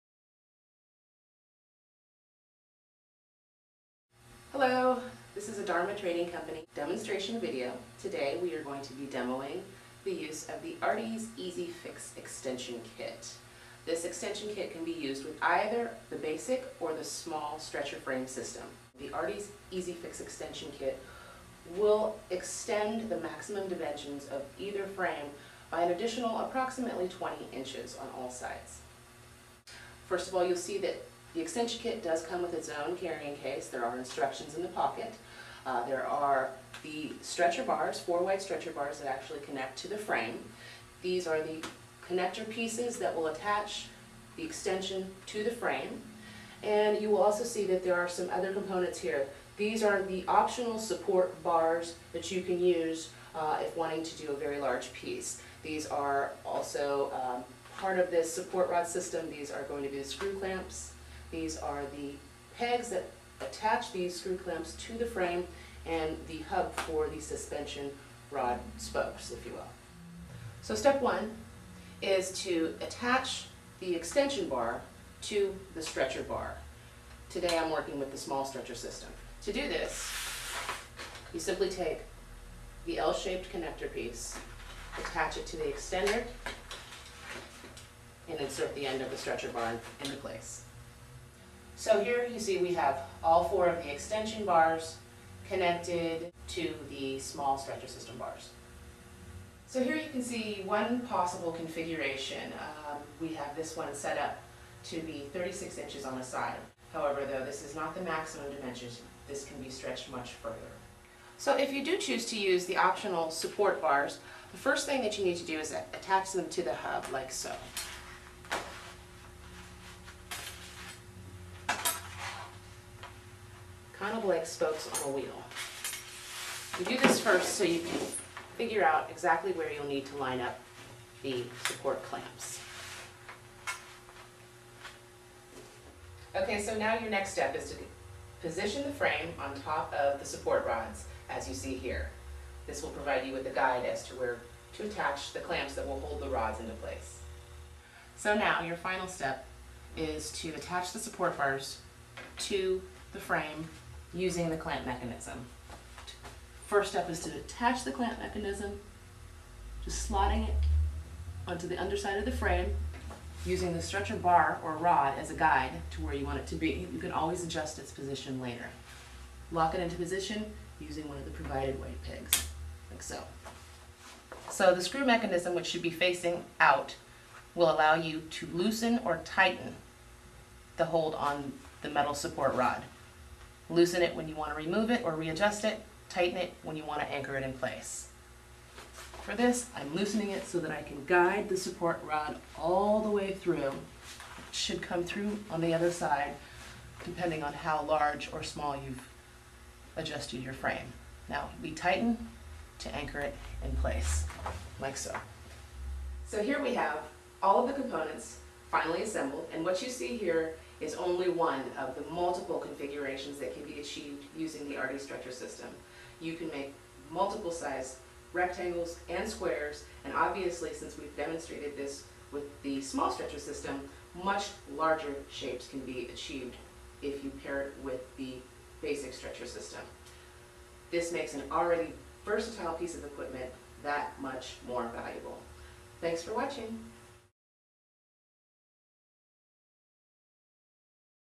SF4 Easy Fix Frame Extender Kit
Dharma Trading Co. product demonstration for the Easy Fix Frame Extender Kit. You can see this kit on our